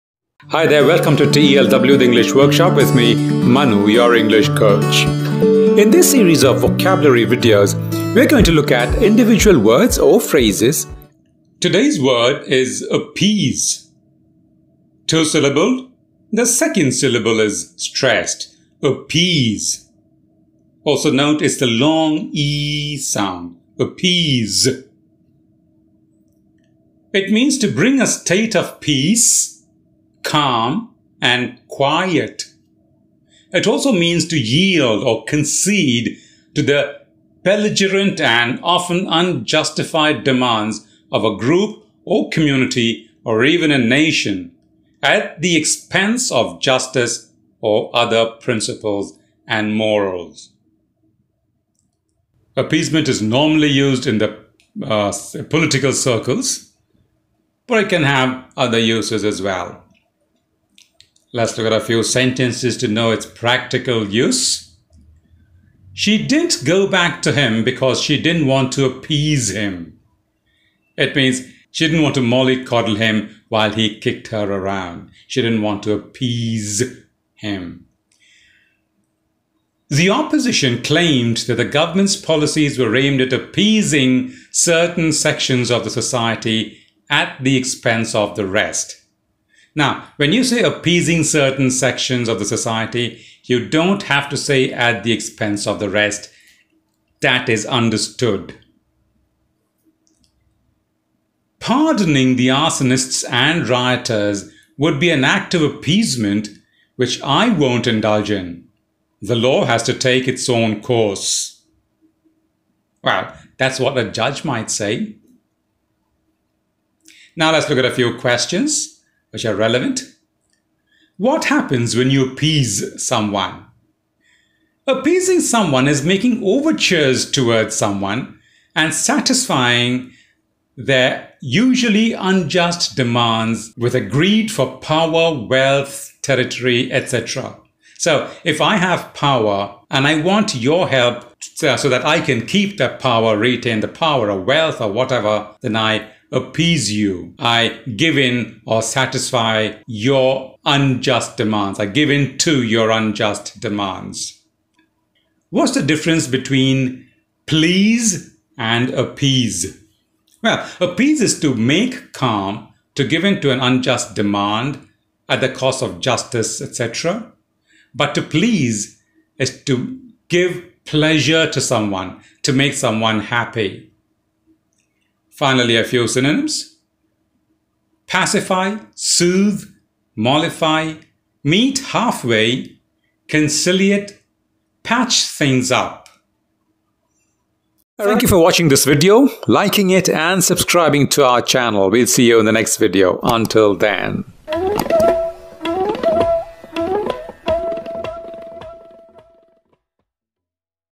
Appease meaning | Learn how to pronounce and use correctly | Synonym of Appease
MEANING:
• To bring to a state of peace, calm, and quiet
• To yield or concede to the belligerent demands of a group or community or even a nation at the expense of justice or other principles and mores.

PRIMARY USE:
• General Can be used in several areas
SENTENCES
She didn’t go back to him because she didn’t want to appease him.
The opposition claimed that the government’s policies were aimed at appeasing certain sections of the society at the expense of the rest.
Pardoning the arsonists and rioters would be an act of appeasement, which I won't indulge in. The law has to take its own course.

QUESTIONS
What happens when you appease someone?
Appeasing someone is making overtures towards someone and satisfying to their usually unjust demands with a greed for power, wealth, territory, &c.
What’s the difference between please and appease?
Appease is to make calm, to give in to unjust demands at the cost of justice or morality, &c. To please is to make happy or give pleasure to someone.
 FINALLY, SYNONYMS

Pacify
Soothe
Mollify
Meet halfway
Conciliate
Patch things up

learn basic english
free english lessons
learn english online
learn english in hindi
best english classes
online english coaching